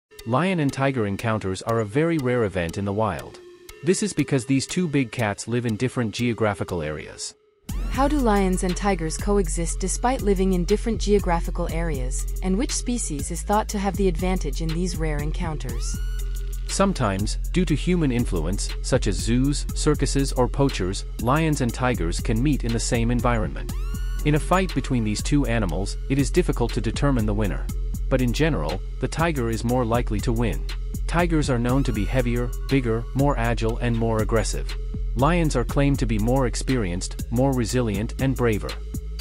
Lion vs. Tiger - Clash of the World's Mightiest Predators: The Ultimate Showdown
Join us in this gripping short video as we witness the intense encounters between two of the world's most powerful predators, the lion and the tiger. Explore the raw strength, agility, and hunting prowess of these apex predators as they face off in the wild. Learn about the unique characteristics and behaviors of each species and gain insights into their epic battles for dominance.